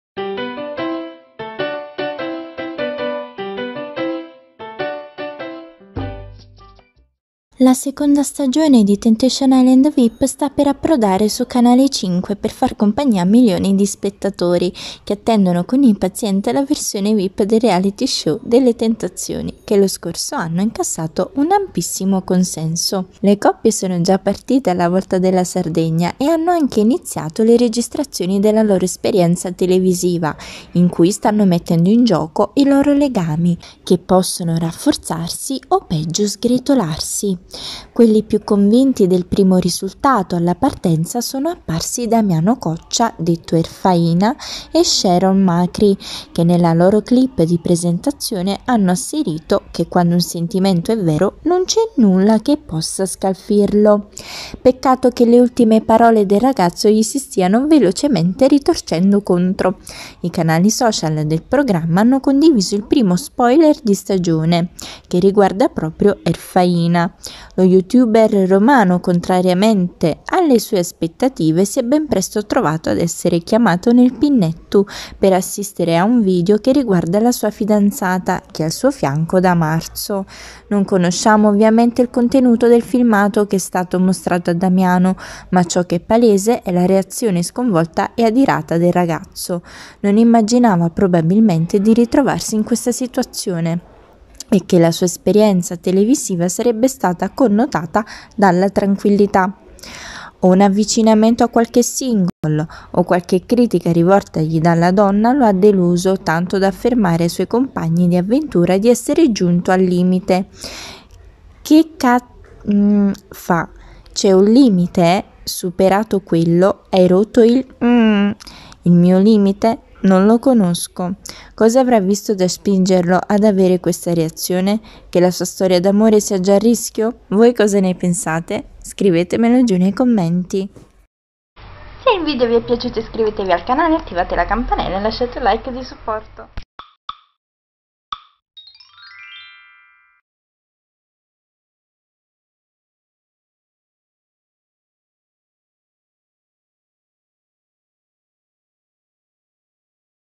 Temptation Island Vip 2: Uno dei fidanzati subito nel pinnettu! Lo spoiler
Sono iniziate le registrazionu di Temptation Island Vip e già ci sono i primi spoiler...